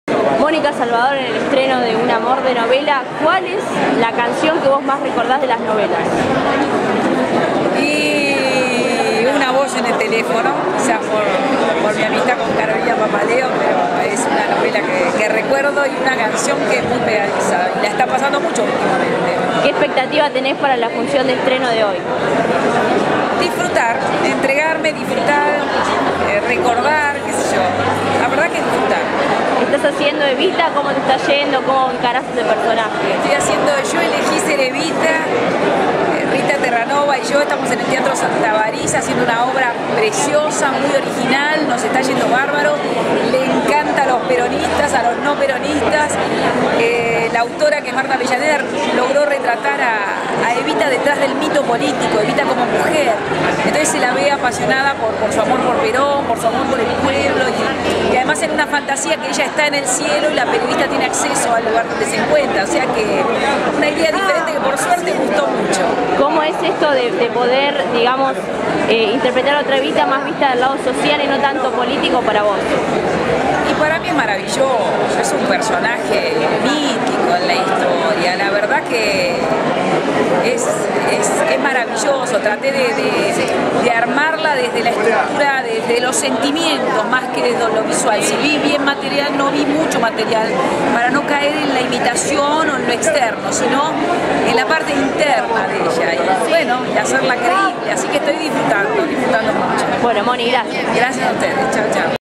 Mónica Salvador - Nota - Estreno "Un Amor de Novela"
Mónica Salvador - Nota - Estreno "Un Amor de Novela". Teatro Nacional, Buenos Aires, Agosto 2011... Nota realizada por Evelyn Camino...